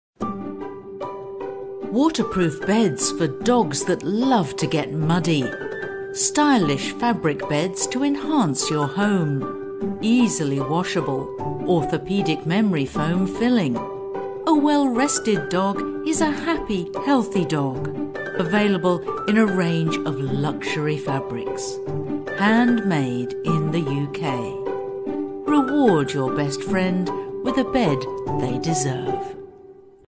Ivy and Duke New Dog Beds - Maggie and Bond
Watch Ivy and Dukes new advert promoting their new dog beds, which are perfect for the winter season! Featuring Maggie and Bond